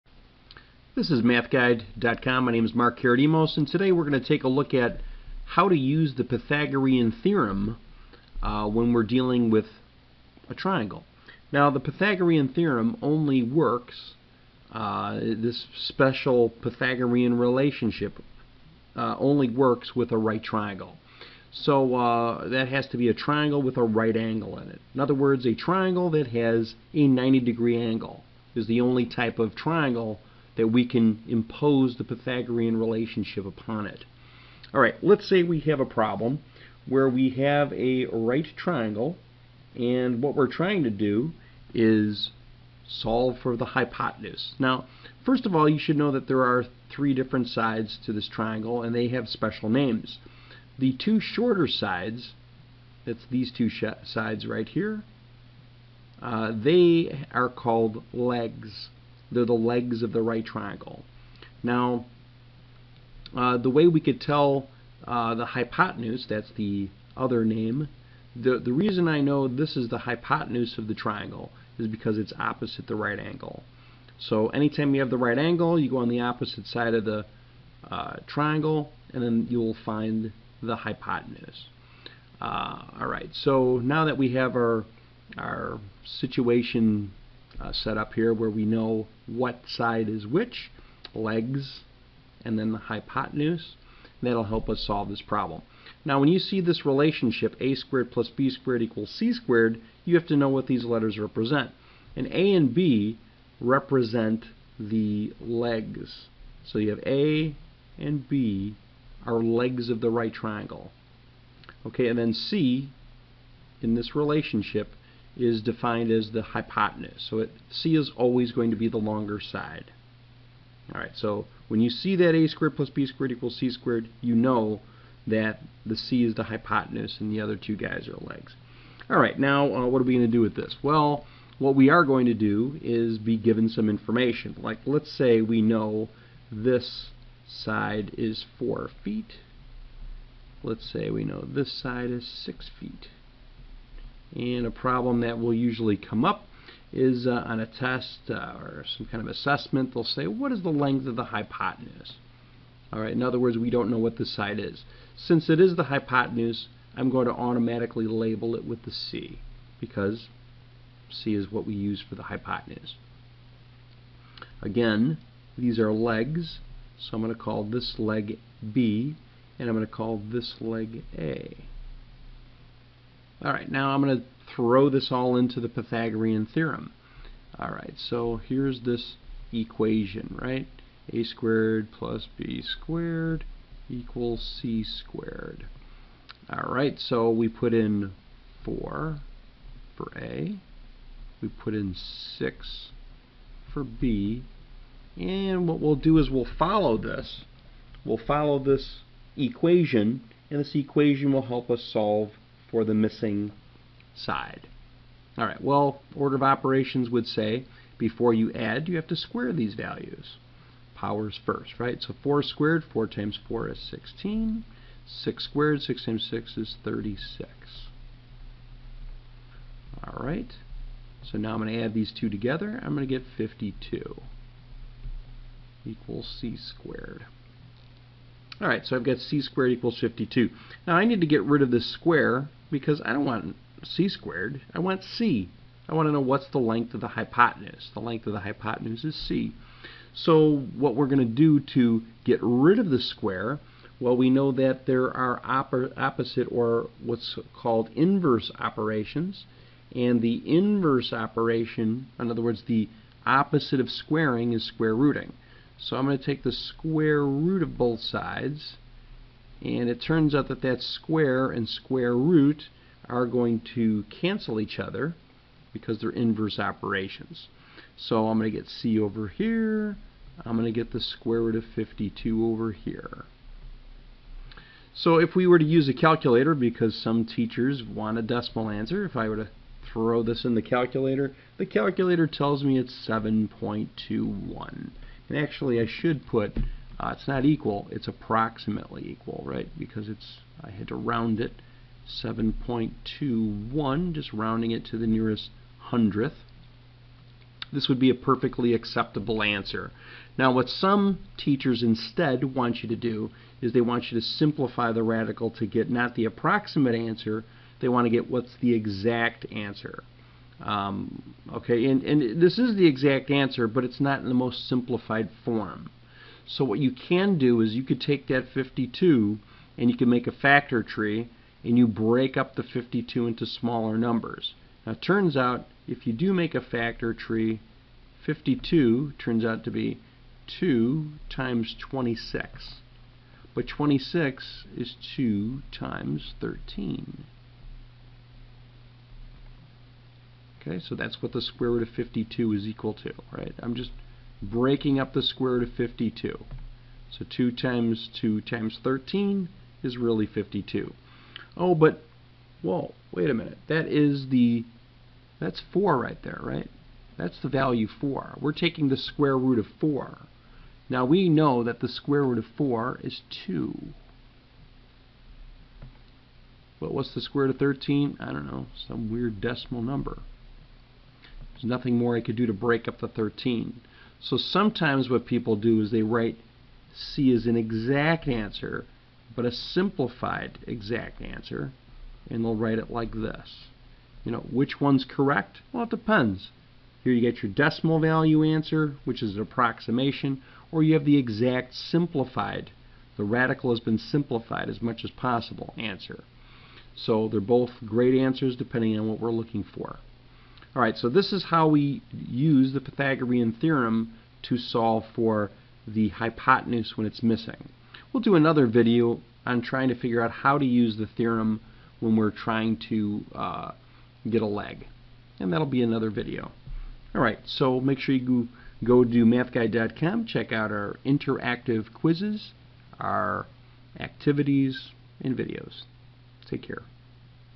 How to Use the Pythagorean Theorem: Hypotenuse
This MATHguide video will demonstrate how to calculate the length of a hypotenuse using the Pythagorean Theorem.